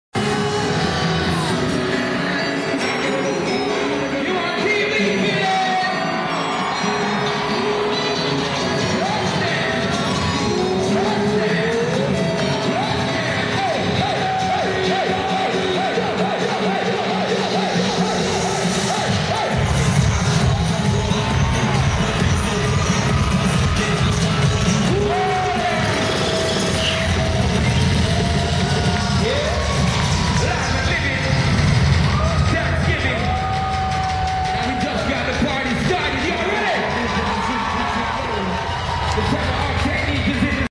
Sean Paul..... Filming HIS FANS.
Just a little Clip of him on stage in Philadelphia 2012